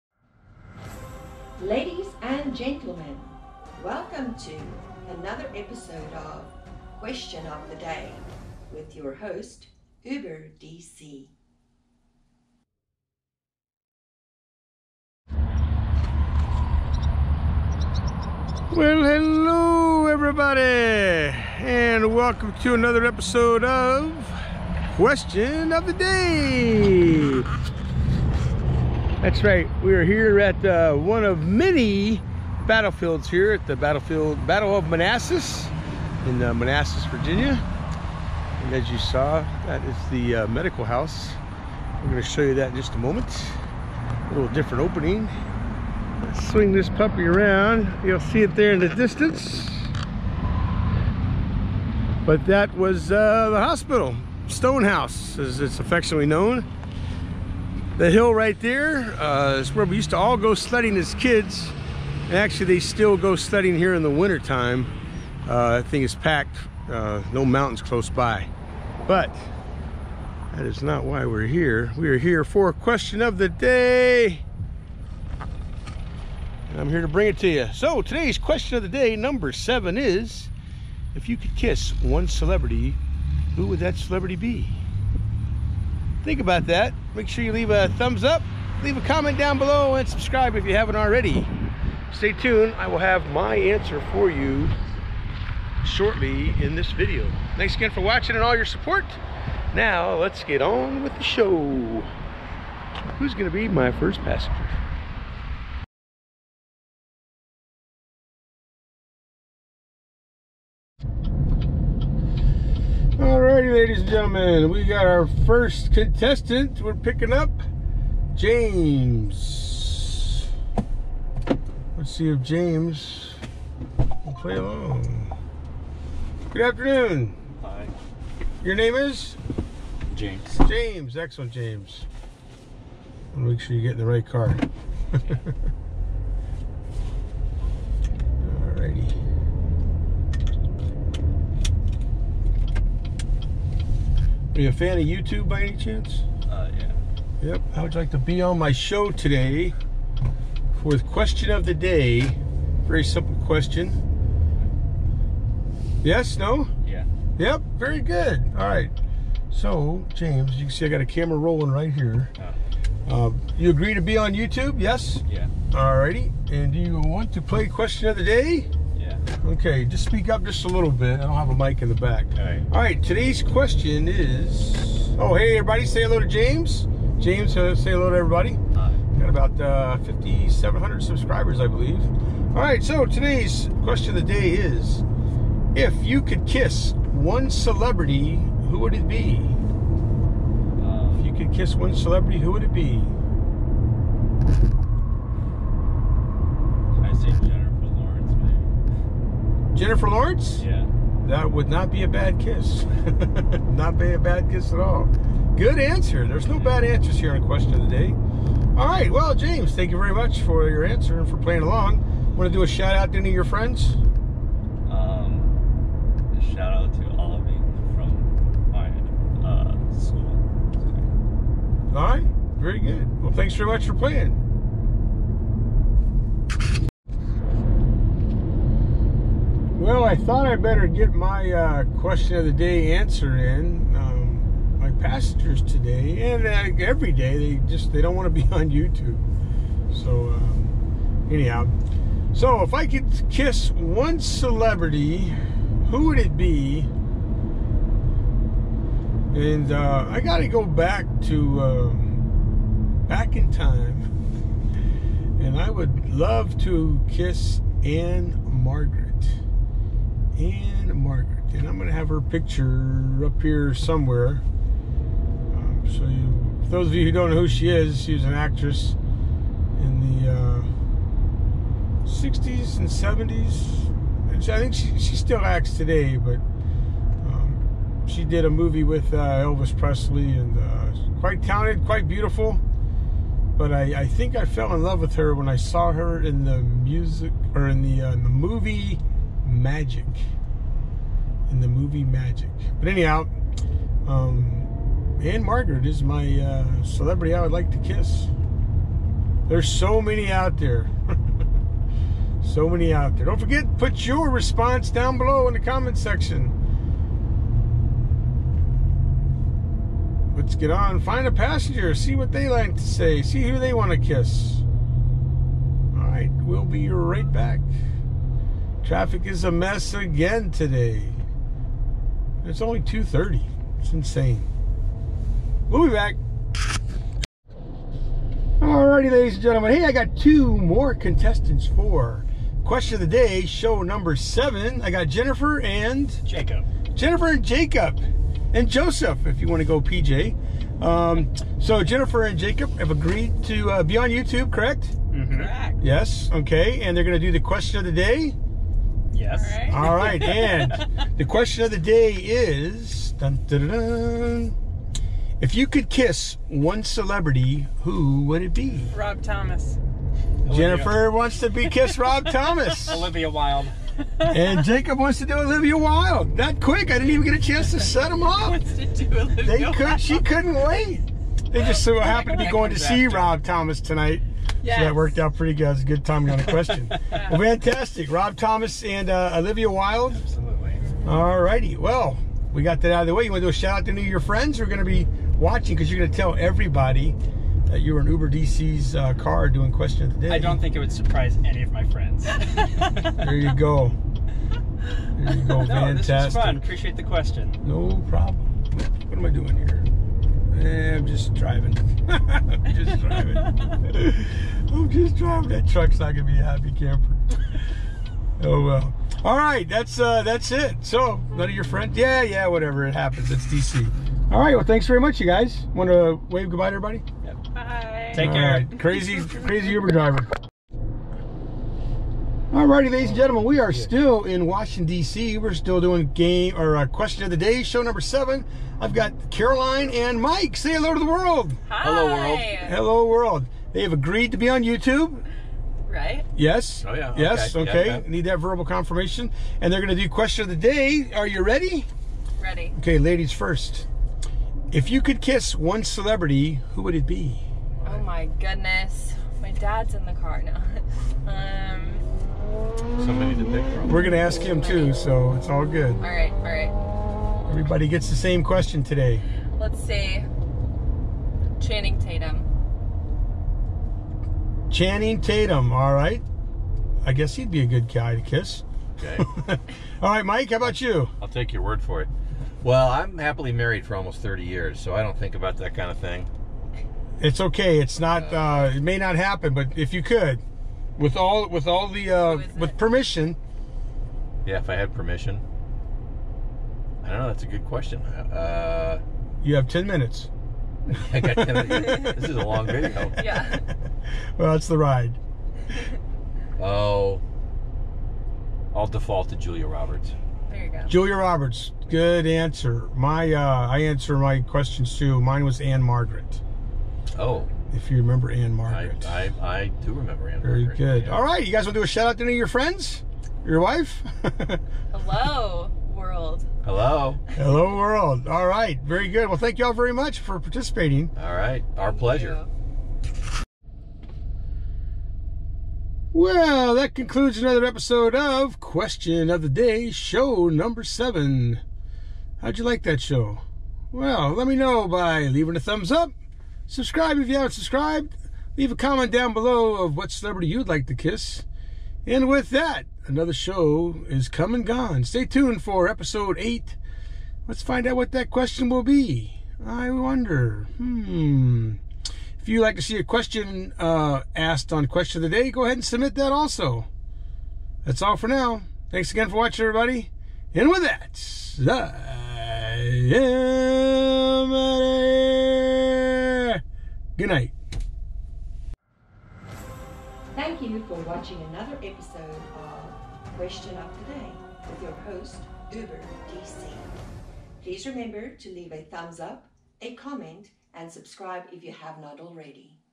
Question of the Day #7 - I Kiss a Celebrity - UBER DC
Question of the Day: If you could kiss one celebrity, who would it
be?
Cameras Used
GoPro Hero Sessions 5
GoPro 7 Black
Phone: iphone8